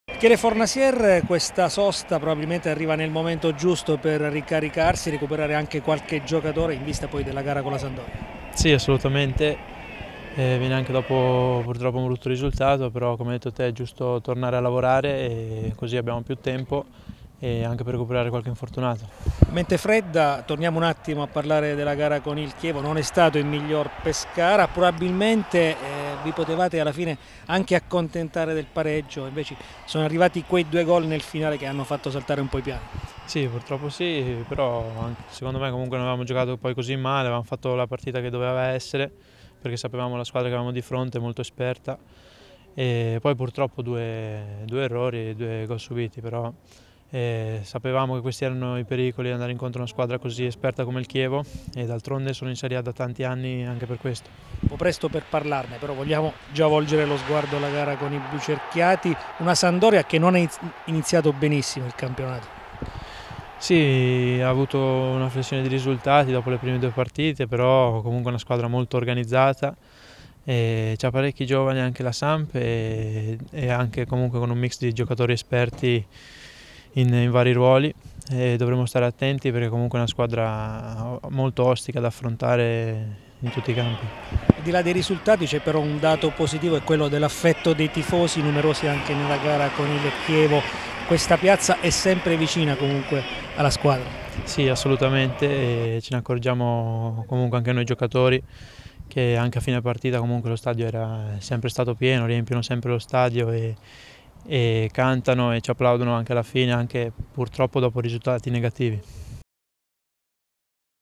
Fornasier sul momento del Pescara
Il Pescara continua a lavorare al campo “Mastrangelo” di Montesilvano dove domani pomeriggio si svolgerà il test con la Primavera. A seguire l'inaugurazione del rinnovato Store ufficiale alla quale parteciperà l'intera squadra con staff tecnico e dirigenziale. Intanto il difensore Michele Fornasier analizza le difficoltà di questo inizio di stagione dei biancazzurri